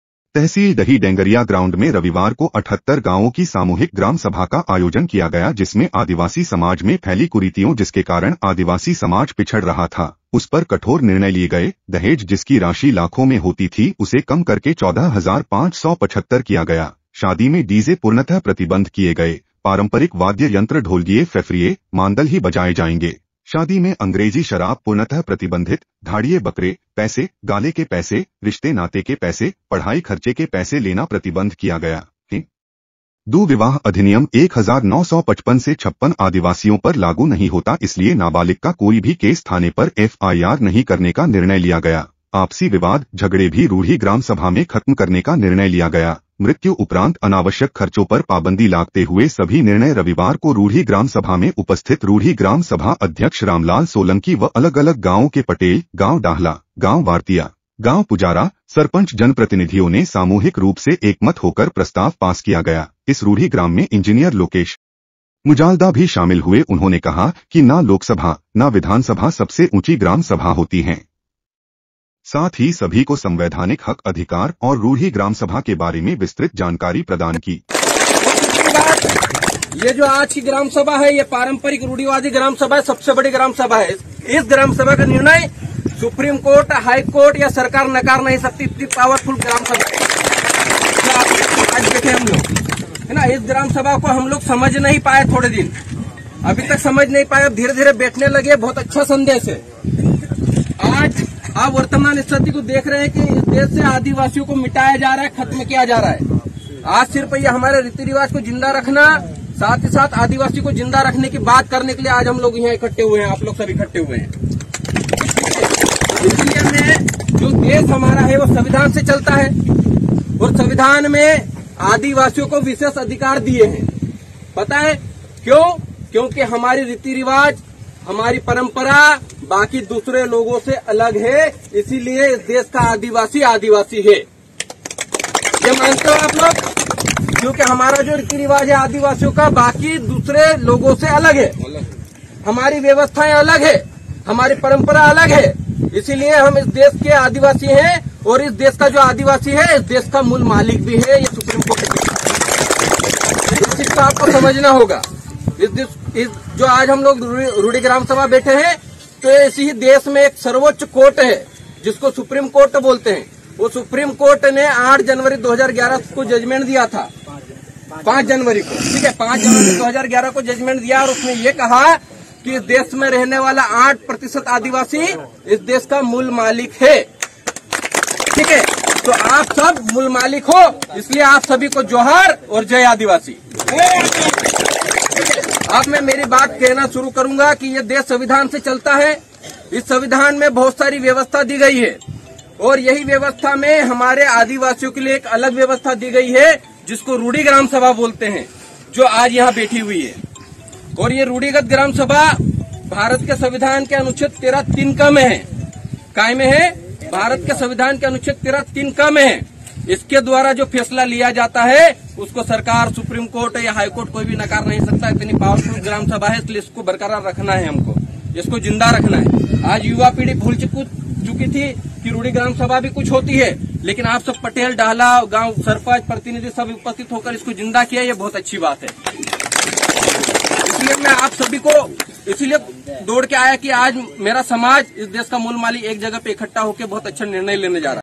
78 गांव के आदिवासी समाजजनों ने लिये कठोर फैसले,अब विकासशील होगा समाज
78 गांव के आदिवासी समाजजनों ने लिये कठोर फैसले,अब विकासशील होगा समाज

*डही मे 78 गांवों की पारम्परिक रूढ़ि ग्राम सभा का फैसला, शादी में डीजे, अंग्रेजी दारू पुर्णतः प्रतिबंध, दहेज की राशि भी निर्धारित*

*ना लोकसभा, ना विधानसभा सबसे ऊँची ग्राम सभा:- इंजी लोकेश मुजाल्दा*

*बारगायां पटेल गुमान के नेतृत्व में हुआ सामूहिक रूढ़ि ग्रामसभा का आयोजन*

डही! तहसील डही डेंगरिया ग्राउंड में रविवार को 78 गांवों की सामूहिक ग्रामसभा का आयोजन किया गया जिसमें  आदिवासी समाज में फैली कुरीतियों जिसके कारण आदिवासी समाज पिछड़ रहा था उसपर कठोर निर्णय लिए गए दहेज जिसकी राशि लाखो में होती थी उसे कम करके 14575 किया गया, शादी में डीजे पुर्णतः प्रतिबंध किये गए पारम्परिक वाद्य यंत्र ढोलगिये फेफरिये, मांदल ही बजाए जाएंगे, शादी में अंग्रेजी शराब पुर्णतः प्रतिबंधित, धाड़िये बकरे, पैसे, गाले के पैसे, रिश्ते नाते के पैसे, पढ़ाई खर्चे के पैसे लेना प्रतिबंध किया गया, हिन्दू विवाह अधिनियम 1955-56 आदिवासियों पर लागू नही होता इसलिए नाबालिक का कोई भी केस थाने पर एफआईआर नही करने का निर्णय लिया गया, आपसी विवाद- झगड़े भी रूढ़ि ग्रामसभा में खत्म करने का निर्णय लिया गया, मृत्यु उपरांत अनावश्यक खर्चो पर पाबंदी लागते हुए सभी निर्णय रविवार को रूढ़ि ग्रामसभा में उपस्थित रूढ़ि ग्राम सभा अध्यक्ष रामलाल सोलंकी व  अलग-अलग गांवों के पटेल, गांव डाहला, गांव वारतिया, गांव पुजारा, सरपंच जनप्रतिनिधियों ने सामूहिक रूप से एकमत होकर प्रस्ताव पास किया गया, इस रूढ़ि ग्राम में इंजी. लोकेश मुजाल्दा भी शामिल हुए उन्होंने संवैधानिक हक अधिकार और रूढ़ि ग्रामसभा के बारे में विस्तृत जानकारी देते हुए कहा कि संविधान के अनुच्छेद 13 3 क रूढ़ि ग्राम सभा सबसे ताकतवर ग्रामसभा है इसलिए सर्वोच्च न्यायालय ने कहा है कि "ना लोकसभा, ना विधानसभा सबसे ऊंची ग्राम सभा" इनके द्वारा पारित किए फैसले को सरकार, या कोई भी कोर्ट नकार नही सकती है इसकी ताकत को समझने की जरूरत है संविधान द्वारा प्रदत्त अधिकारों के बारे में आदिवासी लोगो को जागरूक करना है और आदिवासी क्षेत्रों में ग्राम सभाओं का गठन करके गांव को मजबूत करना तथा गांव के निर्णय गांव में ही ग्रामसभा के लोग तय करें ऐसी व्यवस्था स्थापित करना है यही ग्रामसभा सर्वोपरि है, इसमे प्रशासन की ओर से डही तहसीलदार, राजेश भिंडे, थाना प्रभारी प्रकाश सरोदे तथा इस ग्रामसभा में बालू चोंगड जिला पंचयात सदस्य, सिदड़ी सरपंच बलवीर, भगावा सरपंच कैलाश, अतरसुमा सरपंच राकेश, नेचुरल कम्युनिटी नवल सोलंकी, रूढ़ि ग्रामसभा अध्यक्ष आयुष बामनिया, गांगपुर रूढ़ि ग्रामसभा अध्यक्ष राकेश सोलंकी, दुर्गेश चौहान, भारता, बारगाया, ठेंचा, जामदा, पिपलुद , चाकल्या , टेमरिया, बोड गांव, वासनली, छाछ कुंआ, दशाना, हतवी, जलवट, करजवानी, कलमी, डिग्वि, गाजगोटा, भगावा, अमजपुरा, थांदला सरपंच पटेल, तिखोला अन्य सभी पटेल व रमेश सोलंकी कवड़ा, प्रदीप ससत्या डही, राजेन्द्र डावर, रमेश डोडवा, राकेश ठाकुर, धरामराय सरपंच अनिल सोलंकी, घाणा सरपंच,  भुरू सोलंकी धरामराय, अर्जुन, दुर्गेश वर्मा सहित अन्य समस्त आदिवासी वरिष्टजन जनप्रतिनिधि उपस्थित रहें!